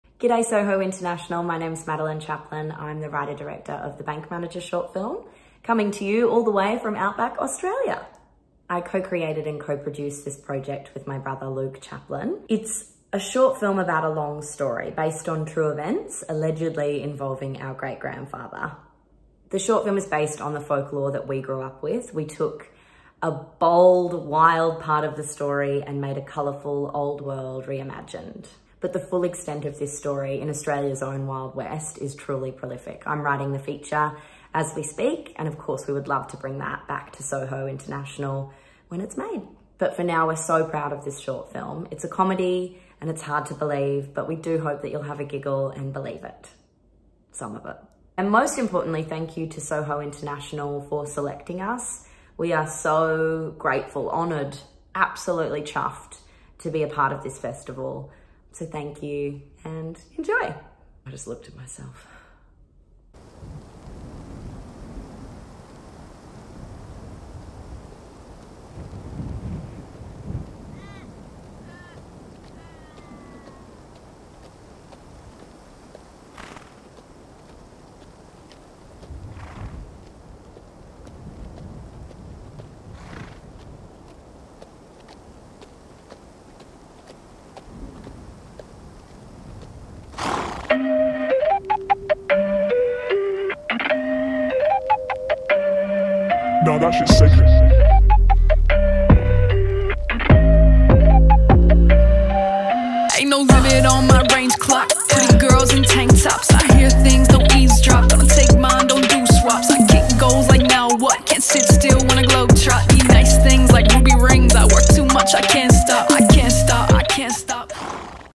MeetTheFilmmakers "THE BANK MANAGER" (Australia)
2023  SOHO International Film Festival
Screening: September 20, 2023

THE BANK MANAGER
Based on true events from 1932 outback Australia. Amidst drought and recession a farmer named Chip (Jason Wilder) devises an elaborate plan to rob his local banks. Yes, plural. But this isn't a gun-slinging Kelly Gang-type affair (no offence). Chip wants to "keep it classy". He assembles a team of locals to help him pull off a sophisticated heist. Phase one: throw a party at the Cloncurry dam and invite the bank manager (Gyton Grantley). The plan is to get him drunk and get him to go swimming so they can borrow his keys to take imprints. But as the bank manager is an uptight rule-follower (with a lot on his mind), will he unwittingly oblige? Can a few maverick cowboys and a woman named Peach really pull off a sophisticated heist? And in a small country town, where everyone knows everyone's business, can they keep the secret?

Director: Madeleine Chaplain
Screenwriter: Madeleine Chaplain, Luke Chaplain
Producer: Luke Chaplain, Madeleine Chaplain
Cast: Gyton Grantley, Jason Wilder, Madeleine Chaplain, Luke Chaplain, Jordan Abbey-Young, Paula Nazarski